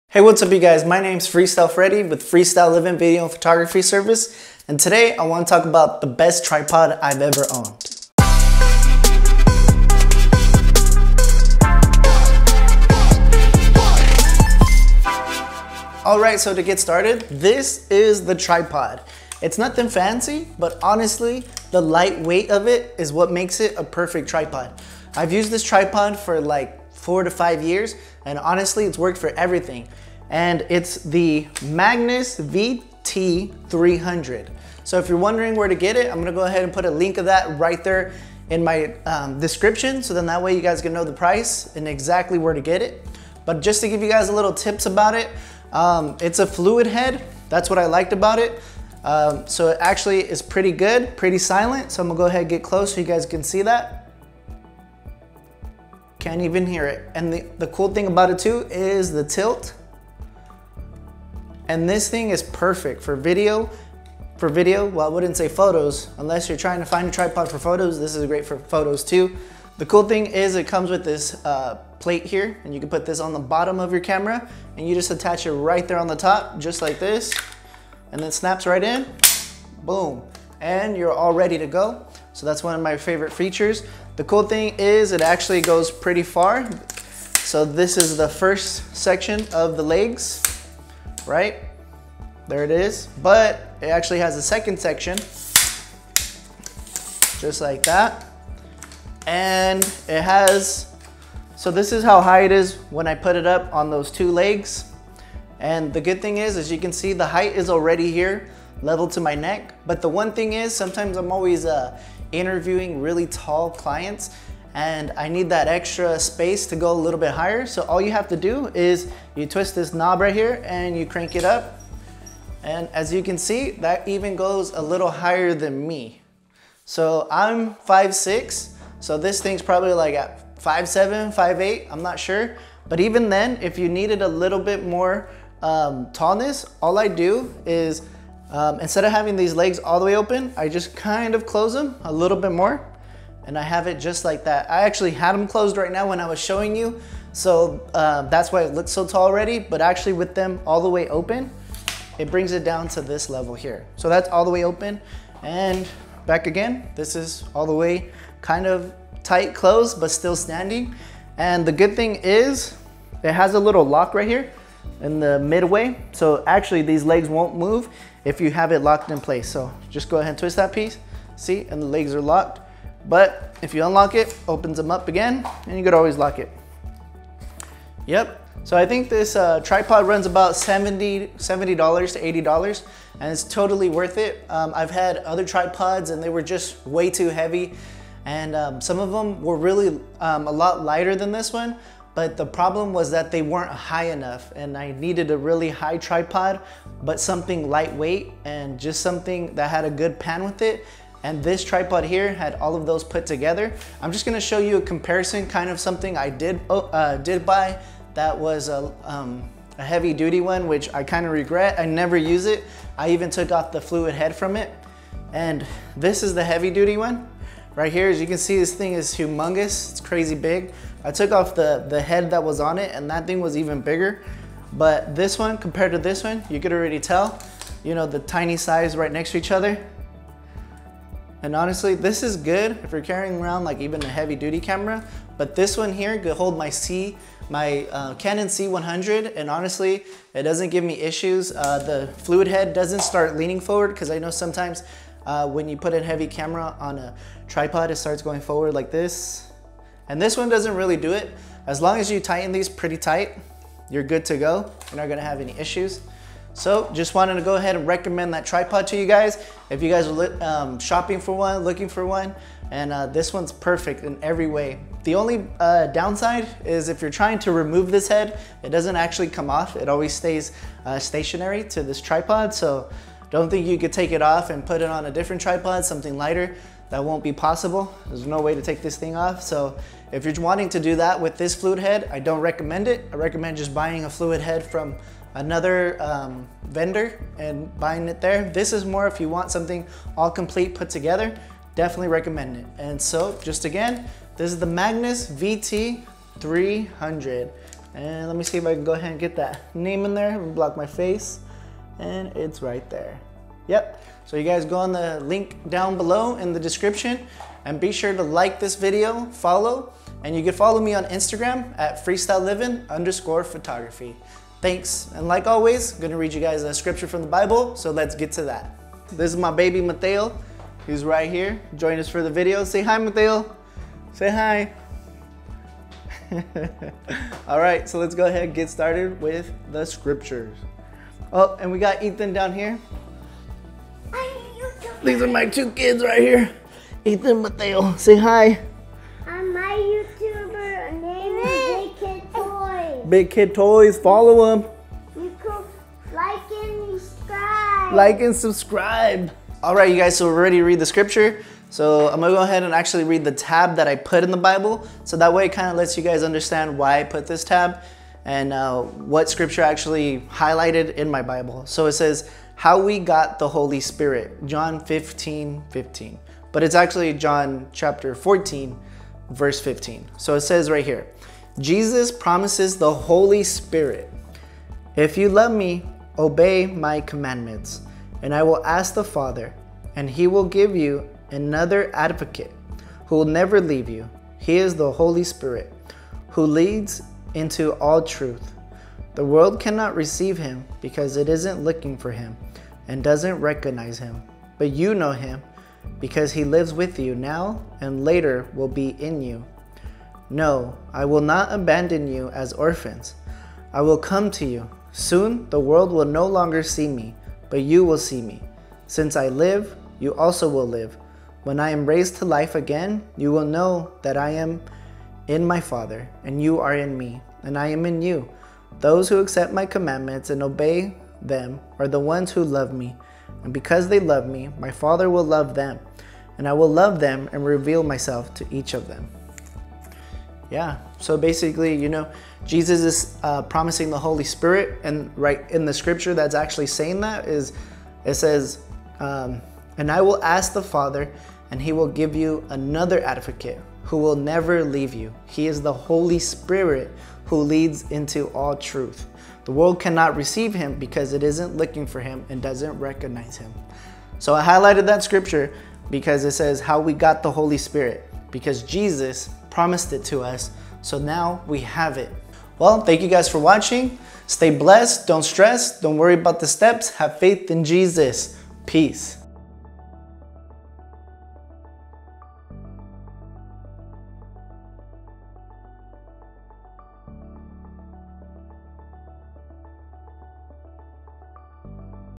THE BEST TRIPOD TO HAVE FOR YOUR BUSINESS! Magnus VT-300
THE BEST TRIPOD TO HAVE FOR YOUR BUSINESS!

LINK TO TRIPOD:

Magnus VT-300, Video Tripod System with Fluid Head, Extends to 64”, Max Load 15 lbs. Mid-Level Spreader, Replaceable Rubber Feet. Plus Quick Release plate, Pan Bar, Carry Case with Shoulder Strap